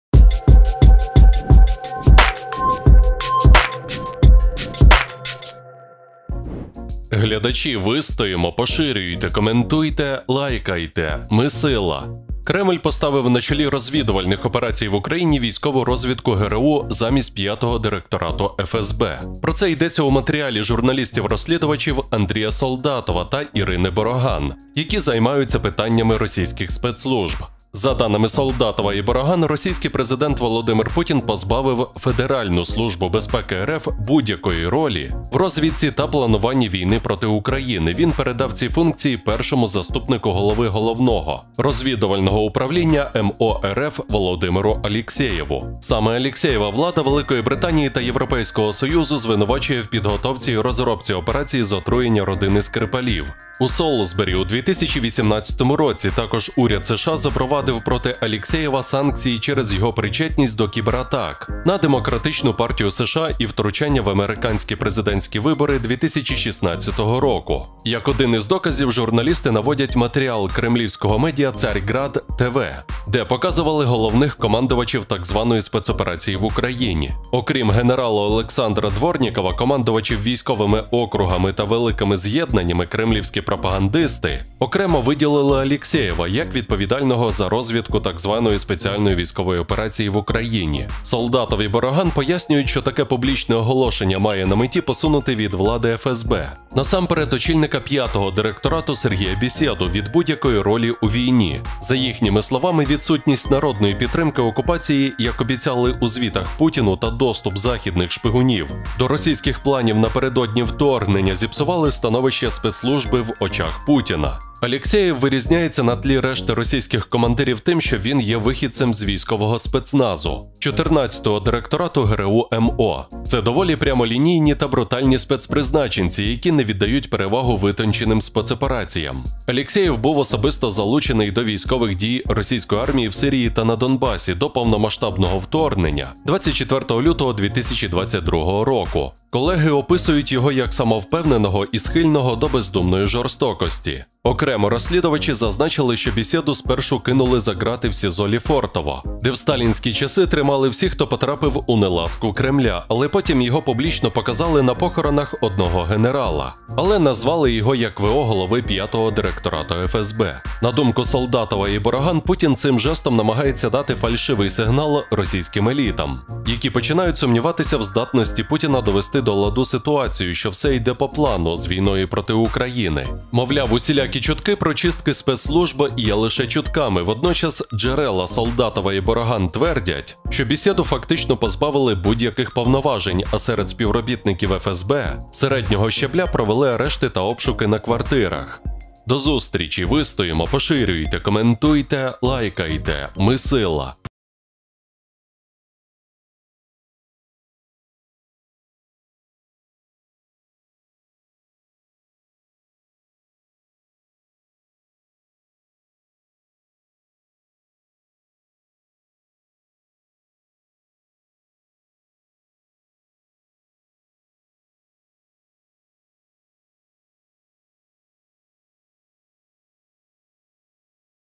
Патрушев переміг! Путін все: ФСБ злито. Почалось - влада вже не його. Прибрали
Кремль поставив на чолі розвідувальних операцій в Україні військову розвідку ГРУ замість П’ятого директорату ФСБ.